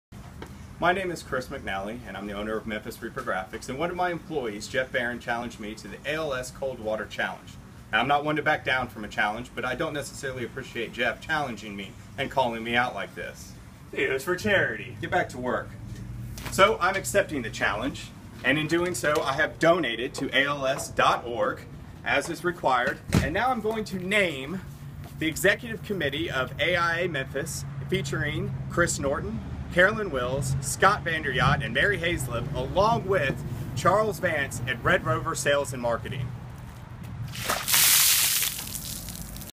ALS Ice Bucket Challenge
Chris McNally at Memphis Reprographics Challenges the AIA Memphis Executive Committee Including Chris Norton, Mary Haizlip, Carolyn Wills and Scott Van Der Jagt. and  Charles Vance at Red Rover Sales and Marketing.